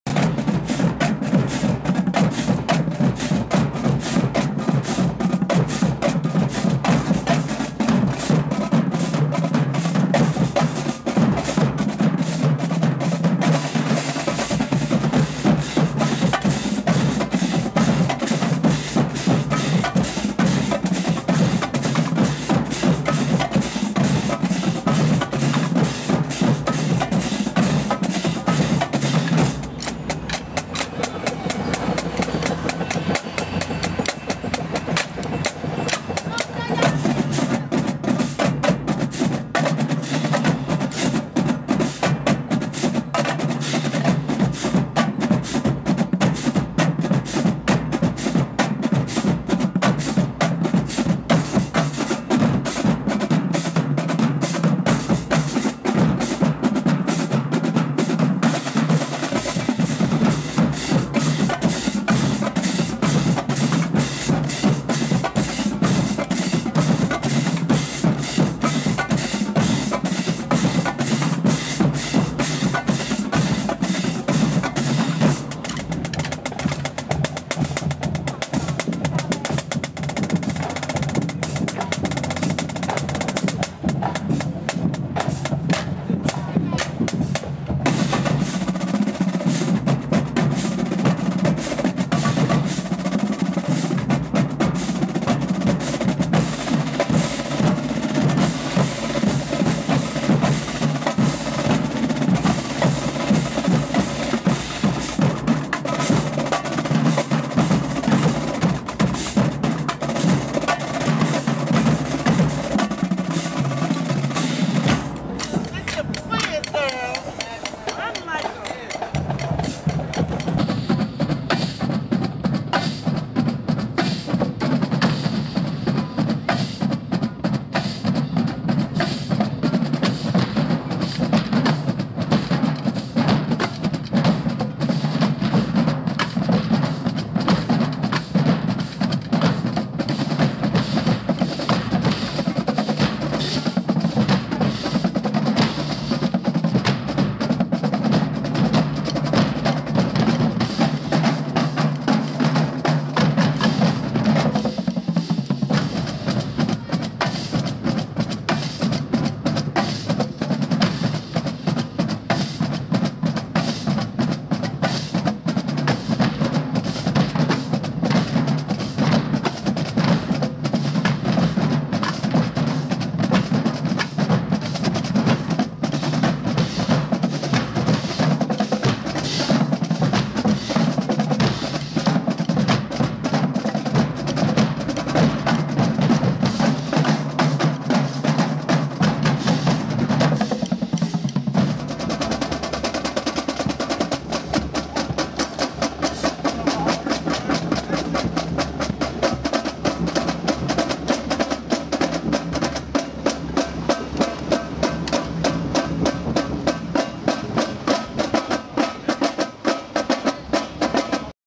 2012 North Carolina A&T University Cold Steel Drumline
Parking Lot warm-up at Drum Line Showdown.